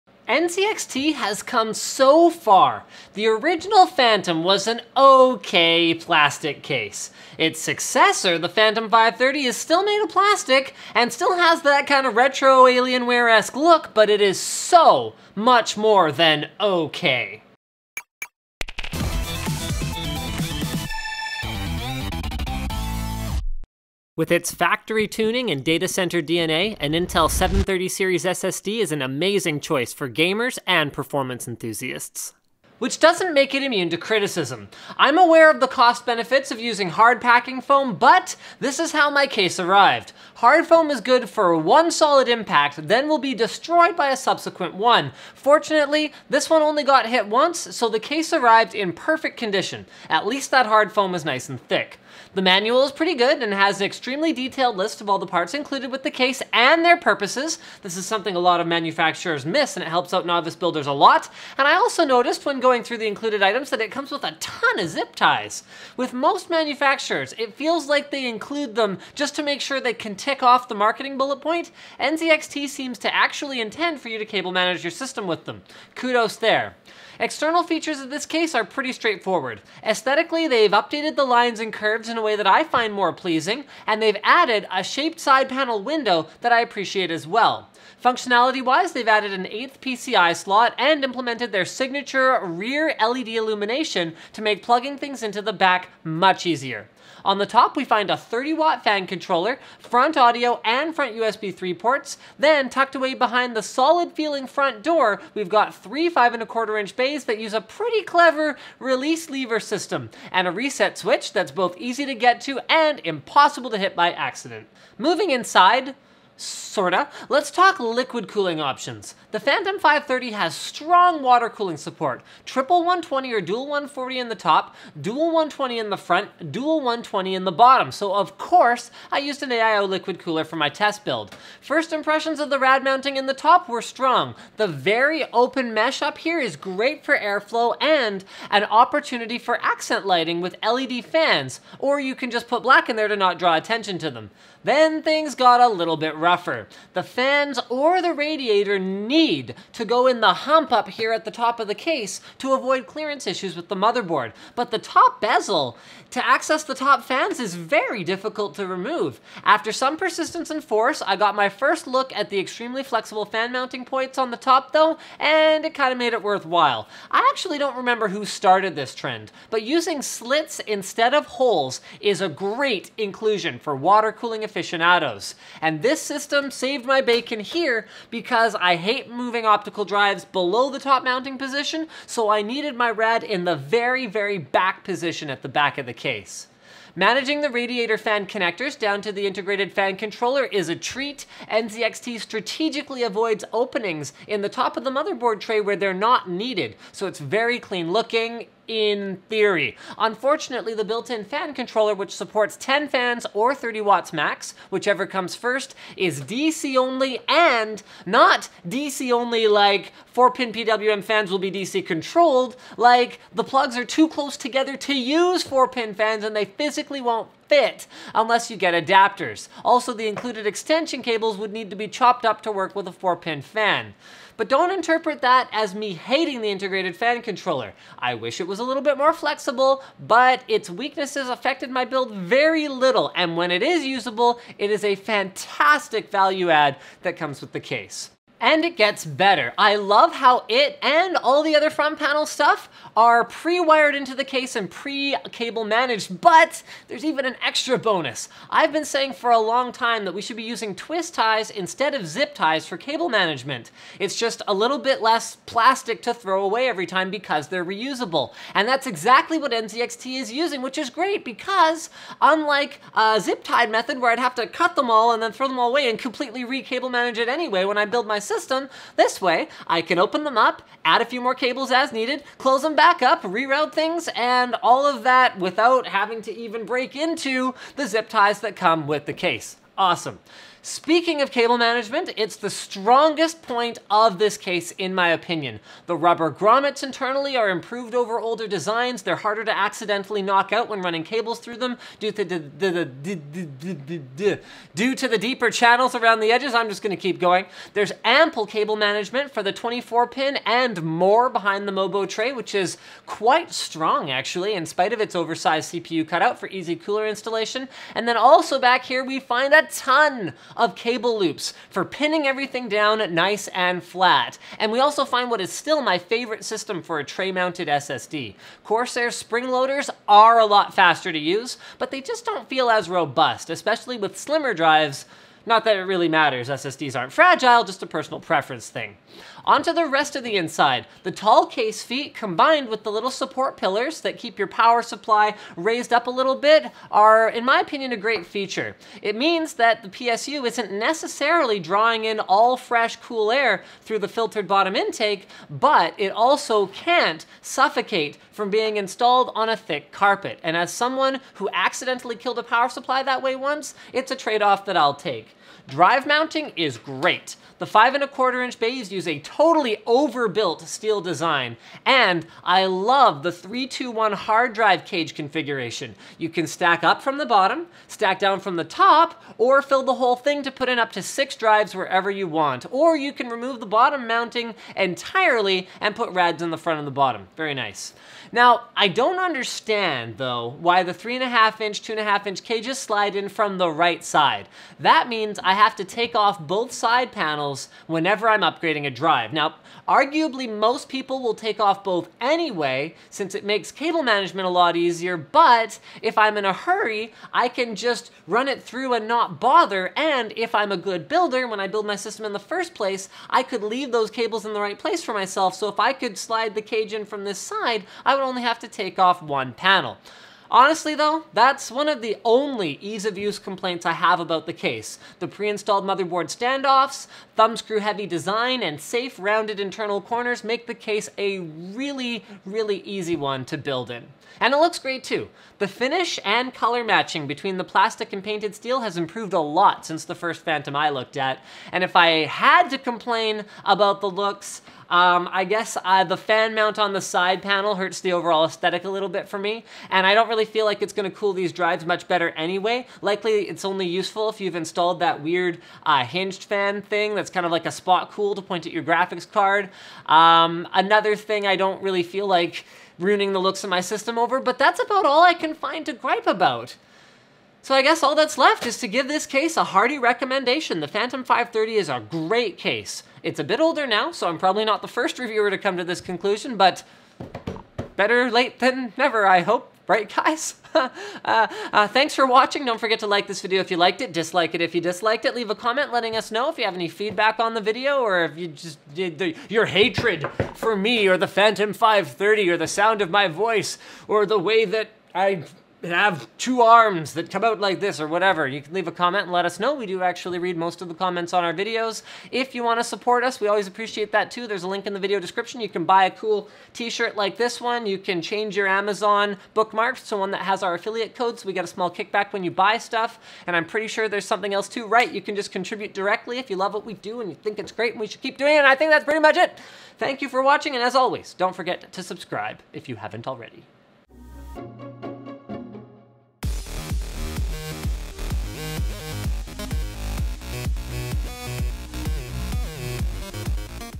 NZXT Phantom 530 Gaming Full Tower Case
The Phantom 530 from NZXT is one of the best designed cases I've ever worked with. I'm not a huge fan of plastic exteriors, but the guts of this bad boy are just. really well thought out.